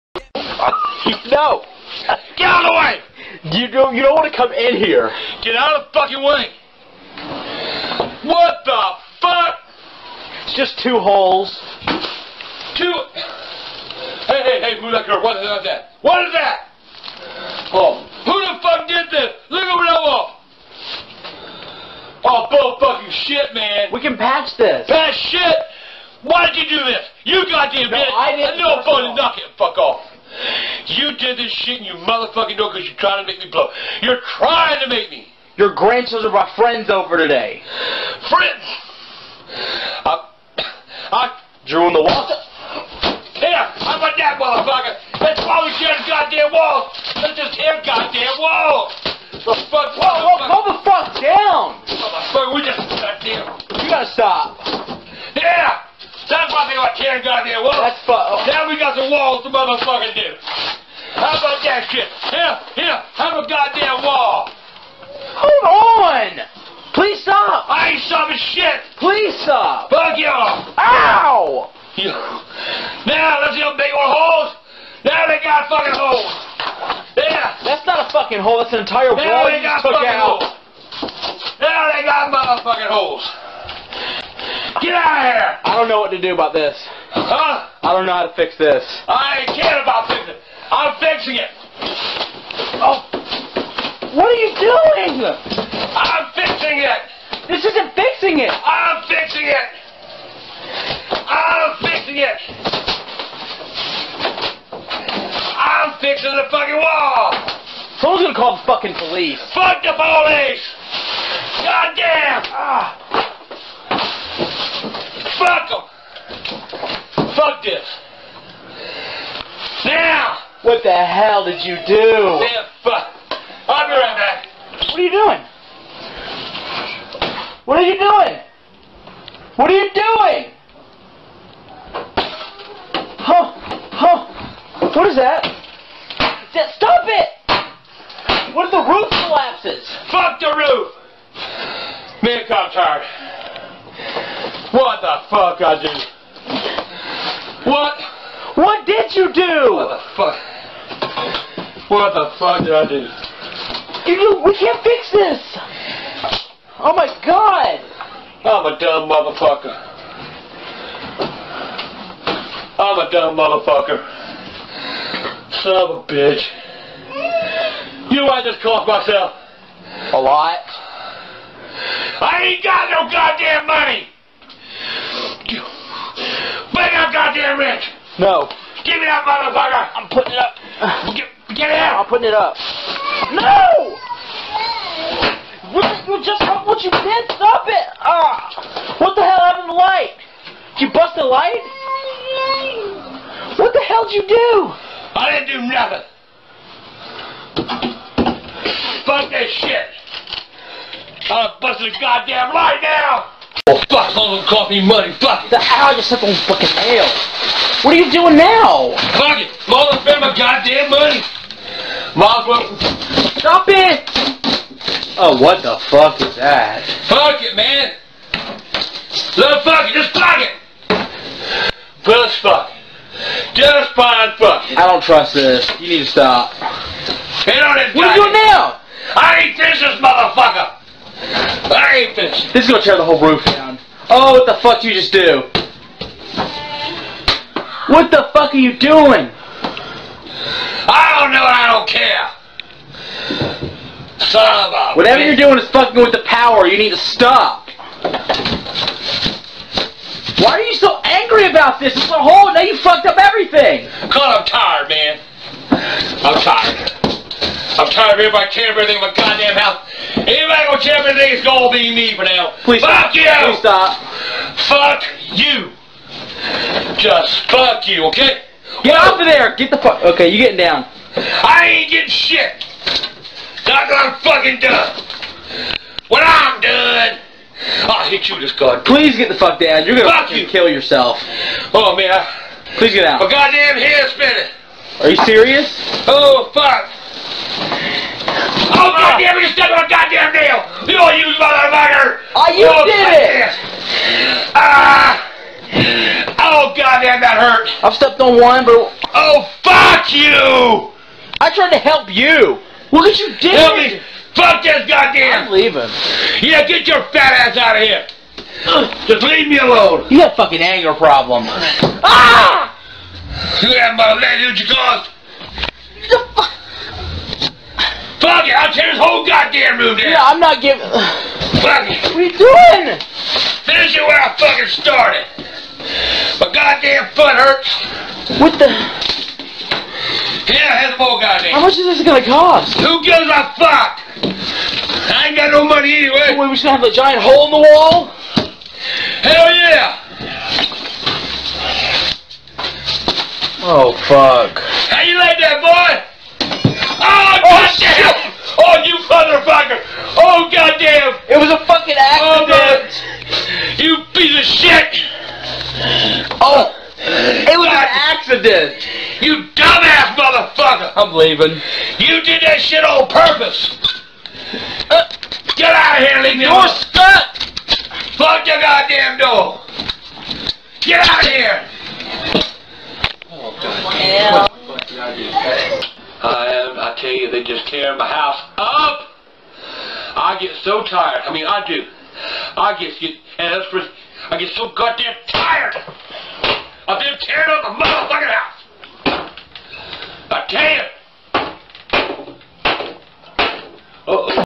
Angry Grandpa: Bringing Down the House! (FULL)
Angry Grandpa finds out what his grandsons and friends had done to the walls in one of the rooms in his trailer, resulting in him tearing them down.